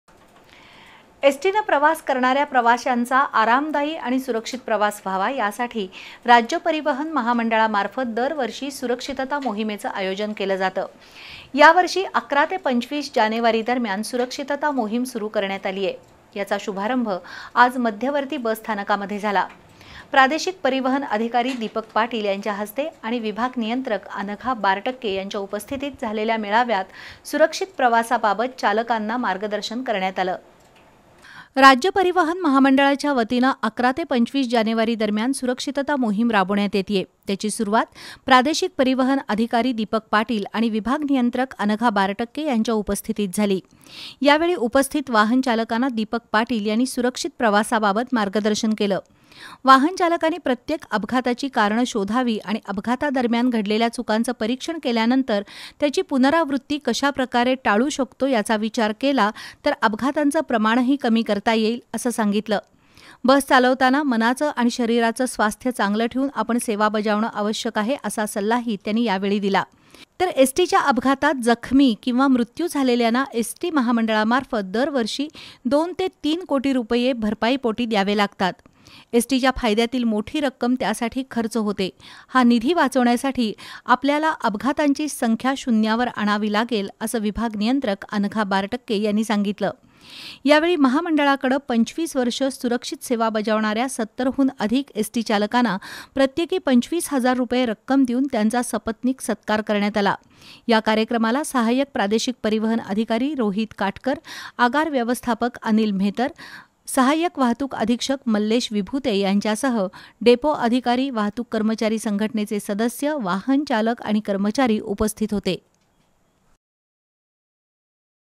परिवहन महामंडळातर्फे सुरक्षितता मोहिमेचा शुभारंभ
वाहन चालकांनी प्रत्येक अपघाताचं परिक्षण करून चुकांची पुनरावृत्ती टाळण्याची गरज, प्रादेशिक परिवहन अधिकारी दीपक पाटील यांचं मत, परिवहन महामंडळातर्फे सुरक्षितता मोहिमेचा शुभारंभ